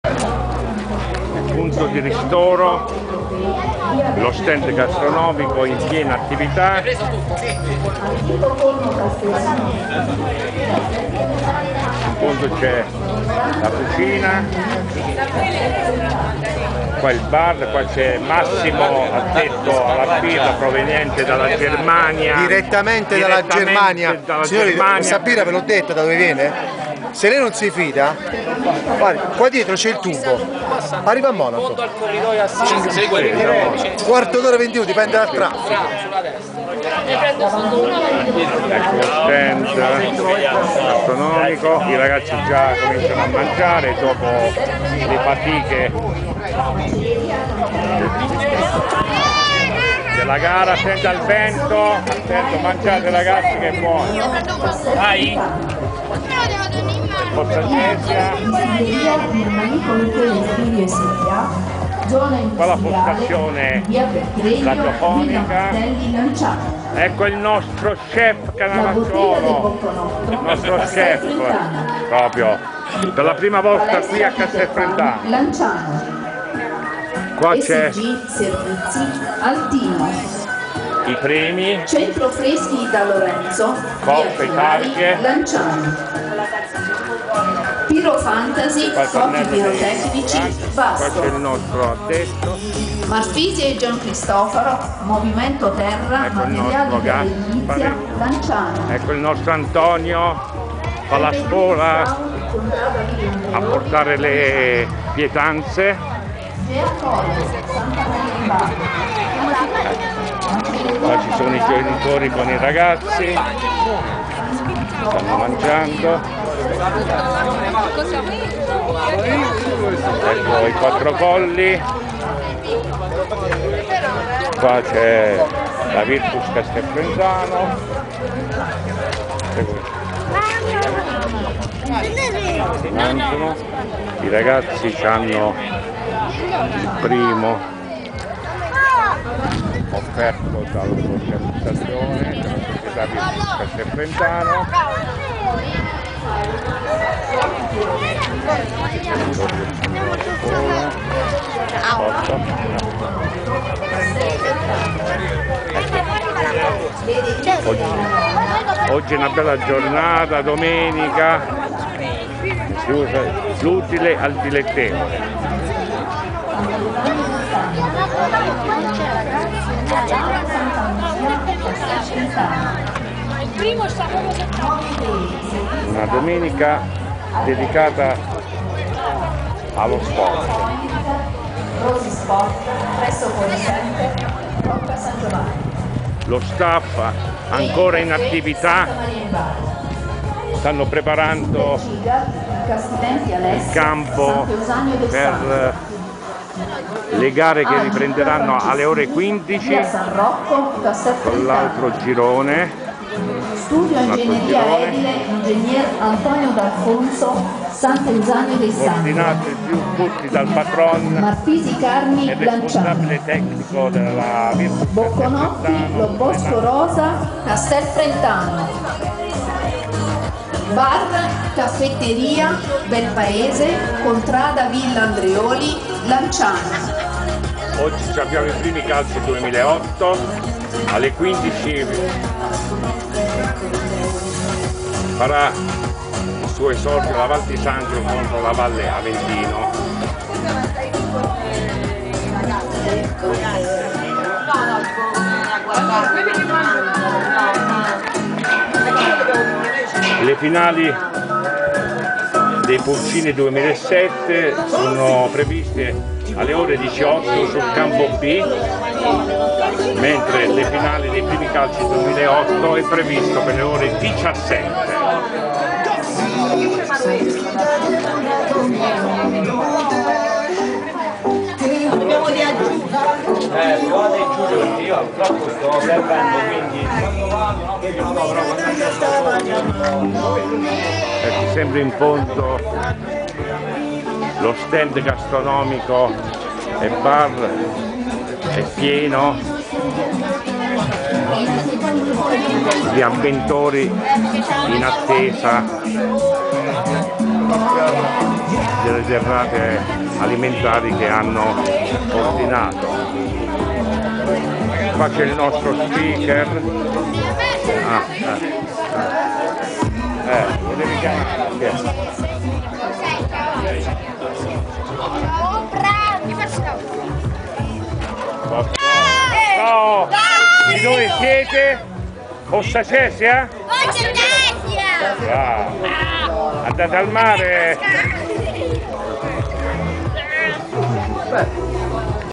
Domenica 18 Giugno 2017 , 4° e ultima Giornata del 3^ Memorial " Massimo SCALINGI
La mattinata svolge al termine . E' l'ora di pranzo per i baby calciatori delle varie Società convenute e i loro Mister , viene offerto il pasto caldo . E' un momento di relax , di confronto tra i vari ragazzi e di nuove amicizie